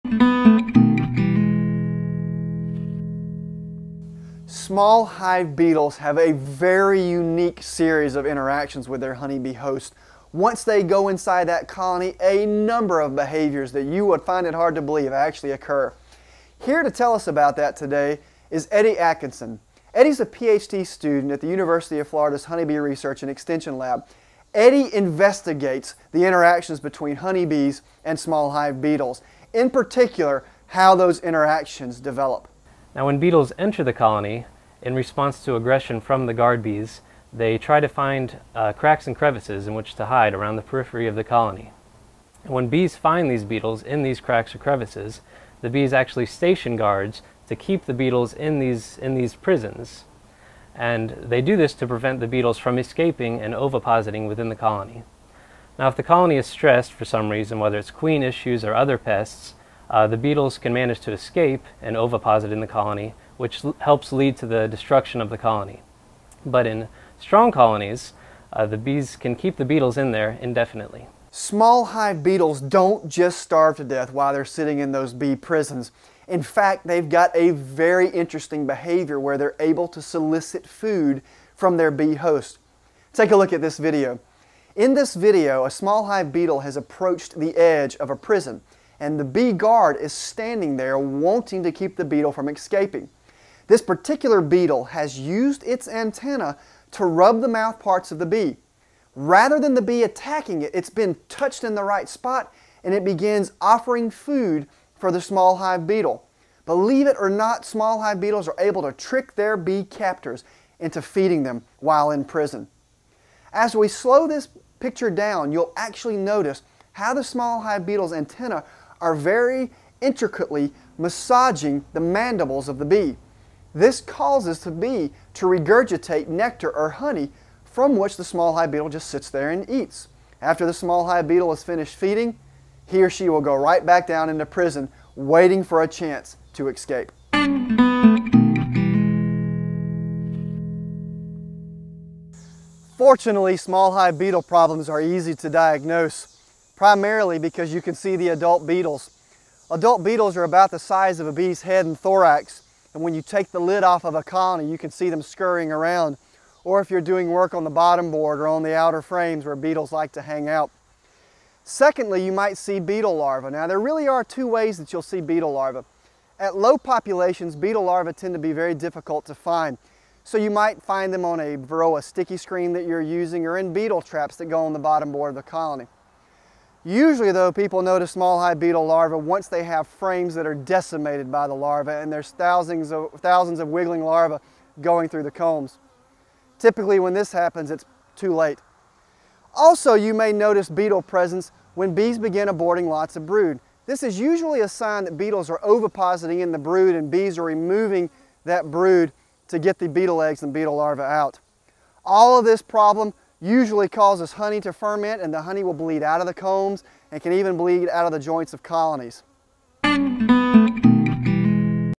Small Hive Beetle Integration and Symptoms
Dr. Jamie Ellis from the University of Florida discusses the interactions and symptoms of small hive beetle infestations in honey bee colonies. This video is part of the compilation: "A Video Field Guide to Beekeeping" produced by the University of Florida Honey Bee Research and Extension lab. To see the video in it's entirety, and additional videos, please